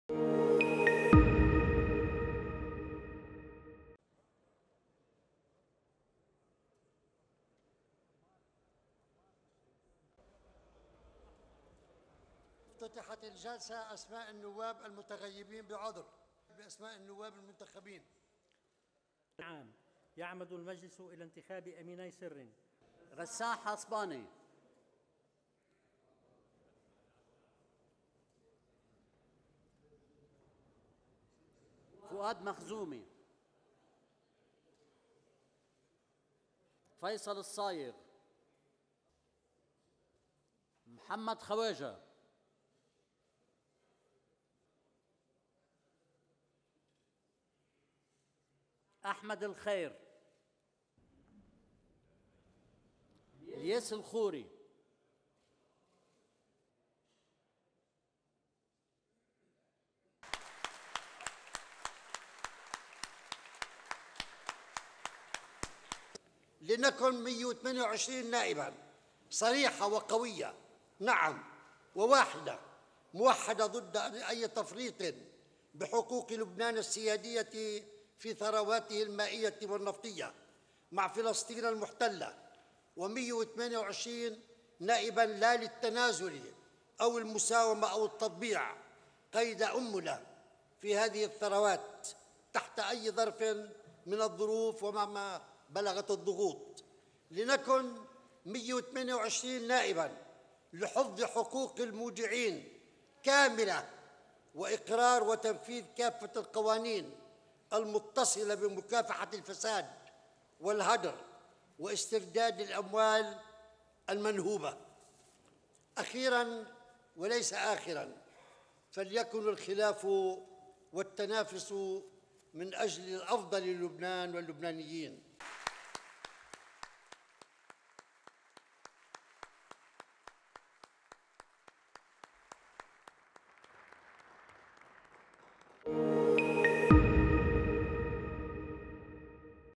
Lebanon - Parliament re-elects Berri as speaker for seventh term.
Lebanon's new legislature narrowly elected veteran Shi'ite Muslim politician Nabih Berri for a seventh term as speaker of parliament in its first session on Tuesday.
He clinched a new four-year term during parliament's opening session with the votes of just 65 of parliament's 128 members. There were 23 blank ballots and 40 were annulled.
Elias Bou Saab, member of Strong Lebanon” bloc, won with 65 votes in the second round of voting against Ghassan Skaff, a newcomer member of parliament who identifies as an independent.
Tuesday's session was the first since the new parliament was elected on May 15th, in the first vote since Lebanon's economic collapse and the Beirut port explosion of 2020.